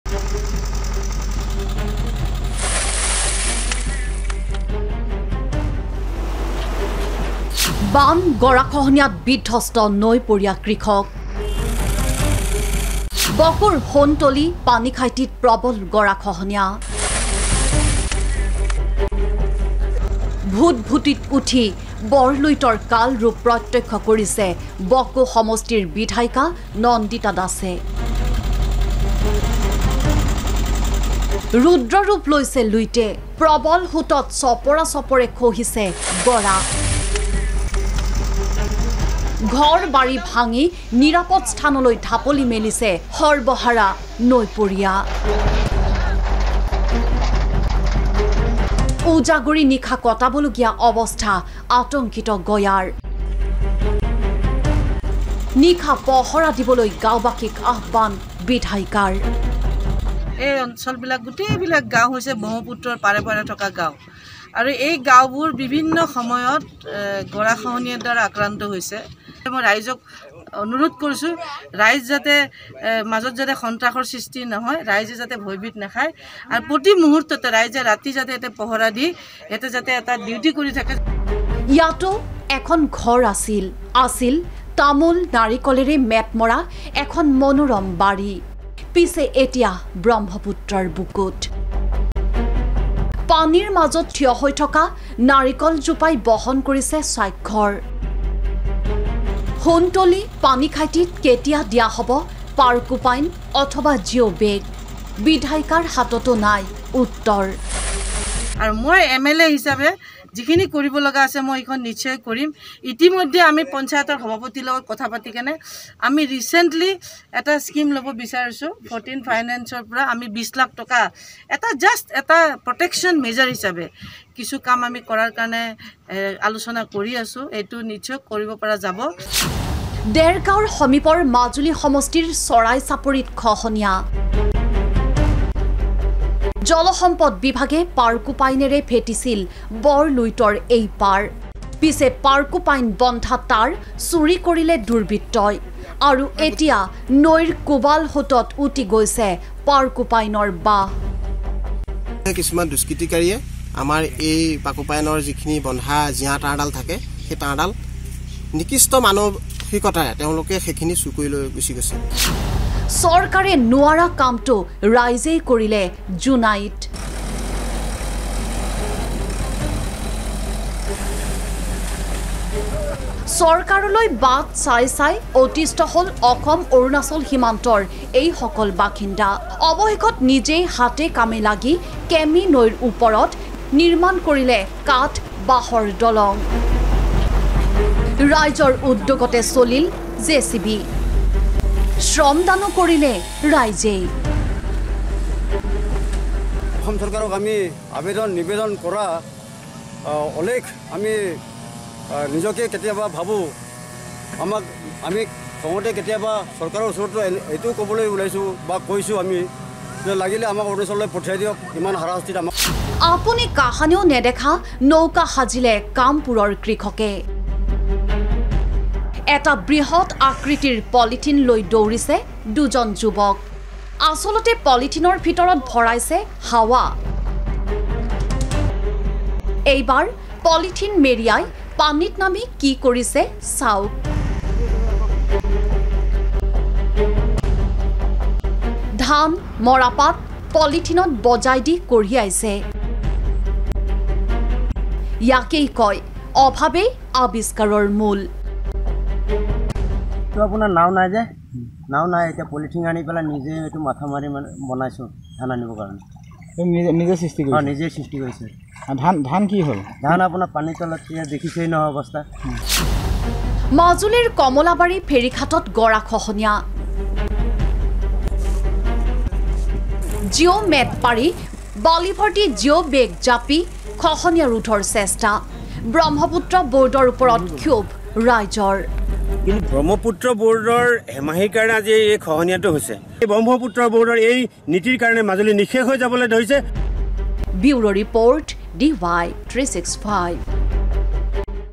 EROSION OVERALL | বান-গৰাখহনীয়াত বিধ্বস্ত ৰাজ্যৰ নৈপৰীয়া কৃষক
বান-গৰাখহনীয়াত বিধ্বস্ত ৰাজ্যৰ নৈপৰীয়া কৃষক। বকোৰ সোণতলী-পানীখাইতিত নৈৰ প্ৰৱল সোঁতত চপৰা-চপৰে খহিছে গৰা। নিজ ঘৰ-বাৰী এৰি নিৰাপদ স্থানলৈ ঢাপলি মেলিছে সৰ্বহাৰা নৈপৰীয়াই। দেৰগাঁৱৰ সমীপৰ মাজুলী সমষ্টিৰ চৰাই চাপৰি, মাজুলীৰ কমলাবাৰী ফেৰীঘাটতো প্ৰৱল গৰাখহনীয়া।